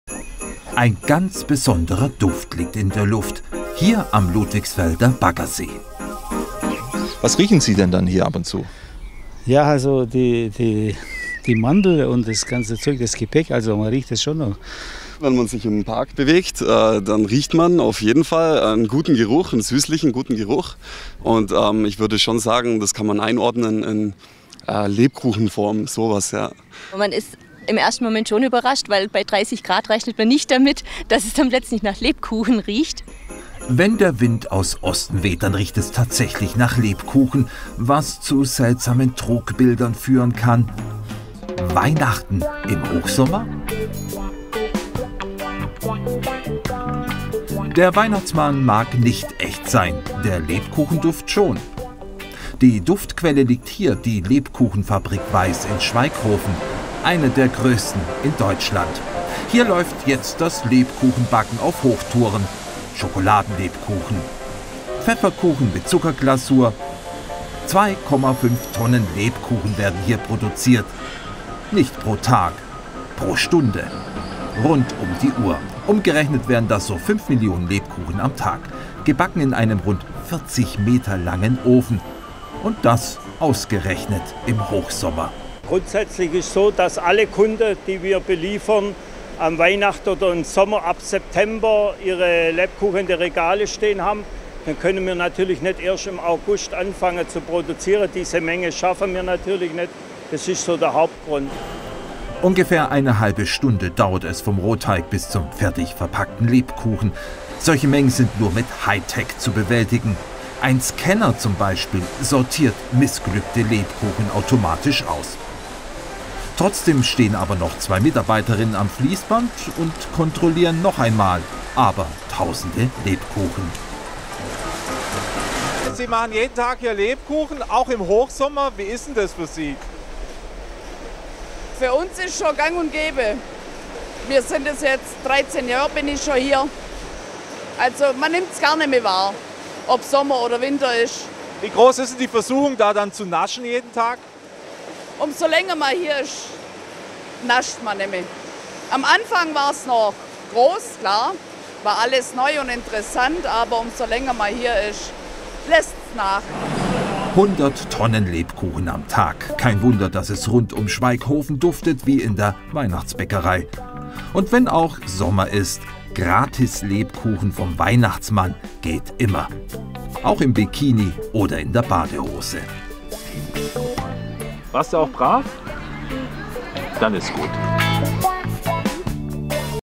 Lebkuchen backen mitten im Sommer
Europas größter Produzent für Lebkuchen sitzt in Neu-Ulm mit Werk in Schwaighofen (bei Neu-Ulm). Hier wird im Dreischichtbetrieb das ganze Jahr über gearbeitet. Wir wollten wissen, wie es dort jetzt im Sommer zugeht.